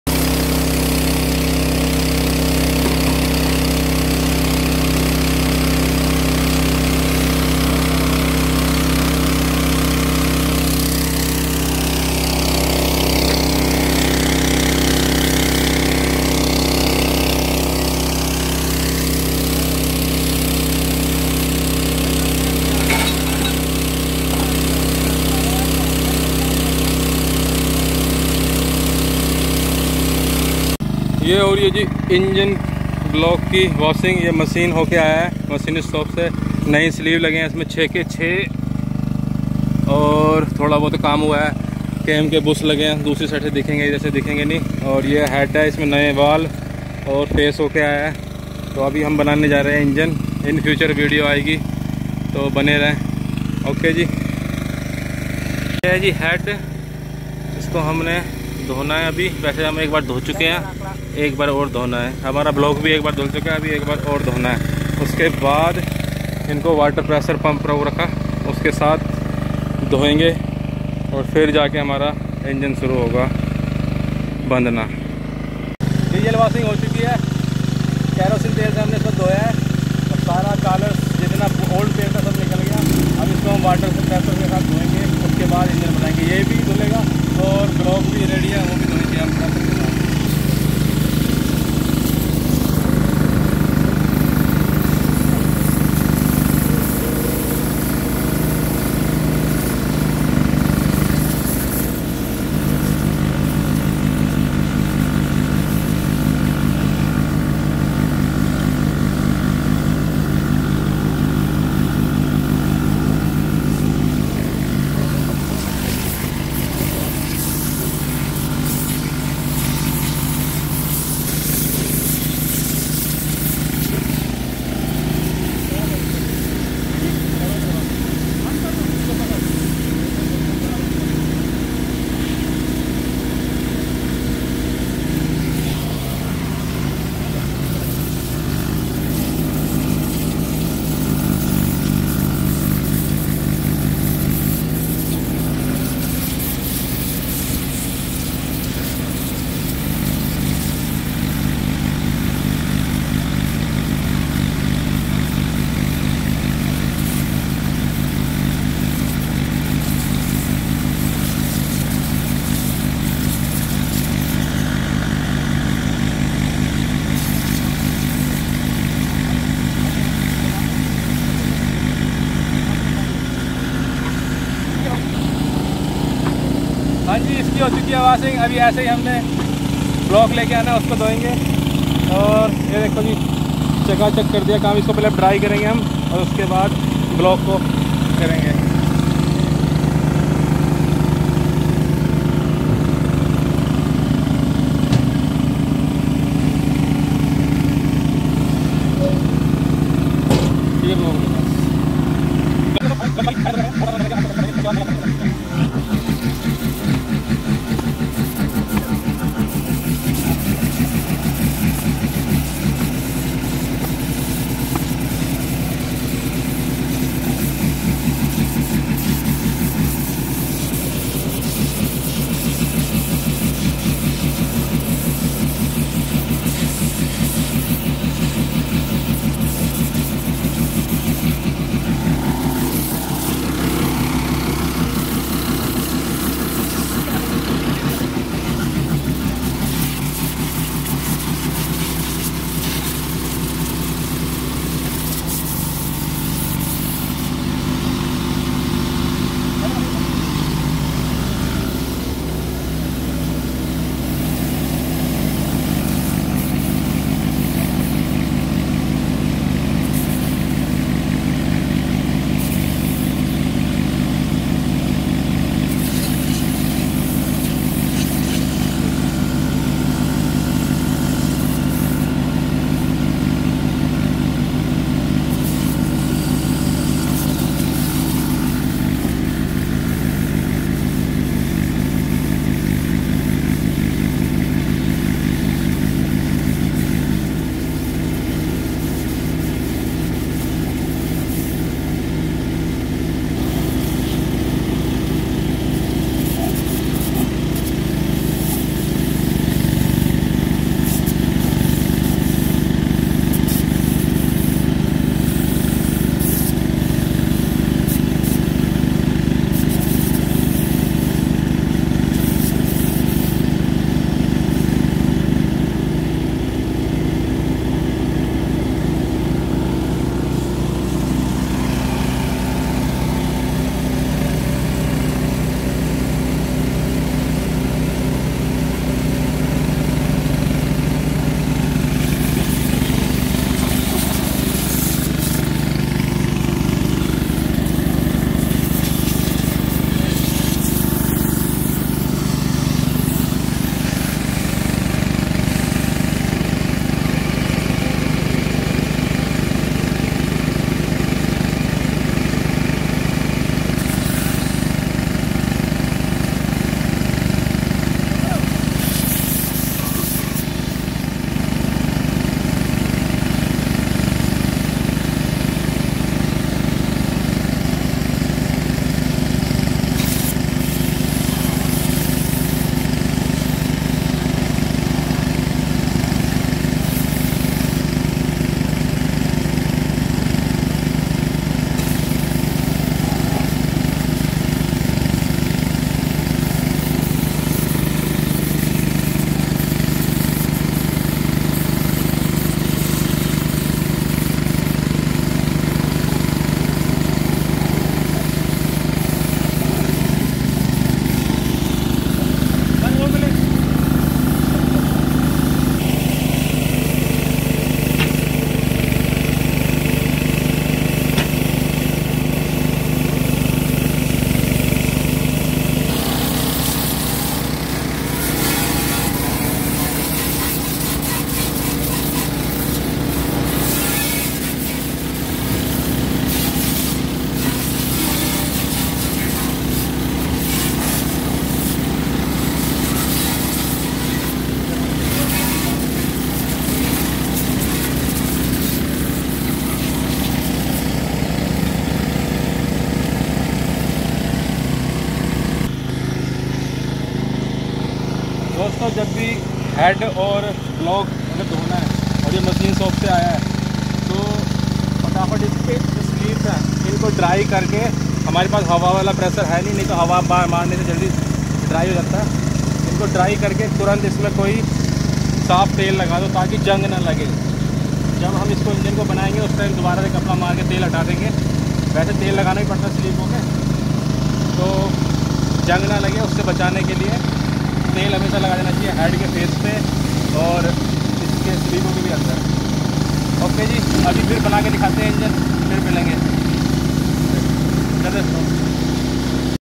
ENGINE OVERHAULING CUMMINS 6BT 5.9 | EXCAVATOR ENGINE REBUILED, HYUNDAI,TATA HITACHI,JCB,SANY,PART 1
THIS IS A FIVE VIDEOS SERIES ABOUT HOW TO OVERHAULING OR REBUILED CUMMINS 6BTAA 5.9 DIESEL FOR MANY 20 TON EXCAVATORS LIKE ... HYUNDAI,TATA HITACHI, JCB, SANY, XCMG ETC...

THIS IS A PART ONE ...